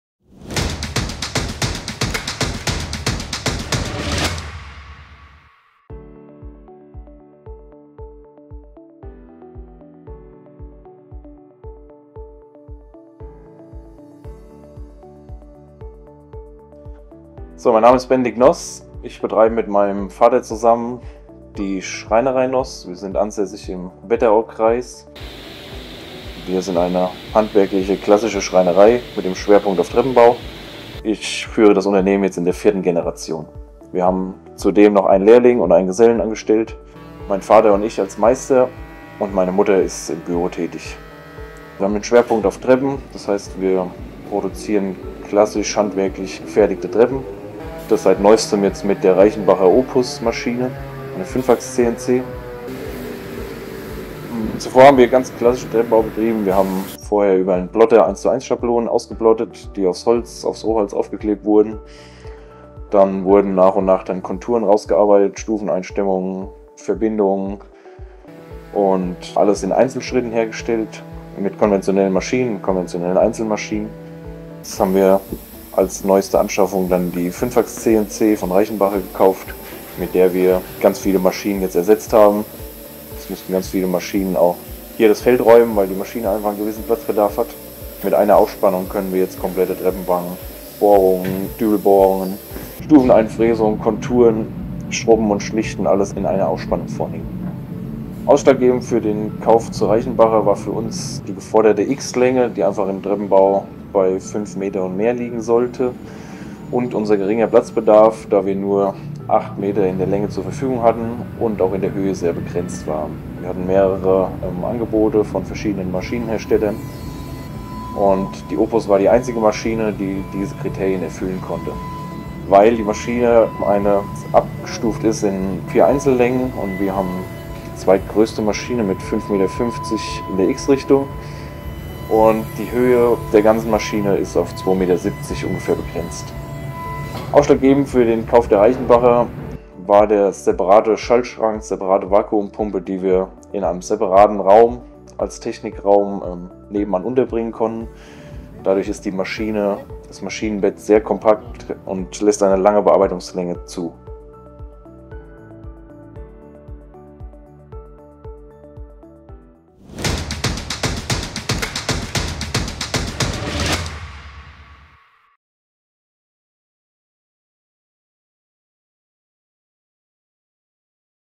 Zu Besuch in der Schreinerei Nos in Hirzenhain
Stefan Nos und Sohn Benedikt haben sich auf den Bau von Holztreppen spezialisiert und die Vier-Mann-Schreinerei auf CNC umgestellt. Die Herausforderung war, eine Maschine zu finden, die in die winzige Werkstatt hineinpasst. Jetzt arbeiten sie mit der Opus von Reichenbacher.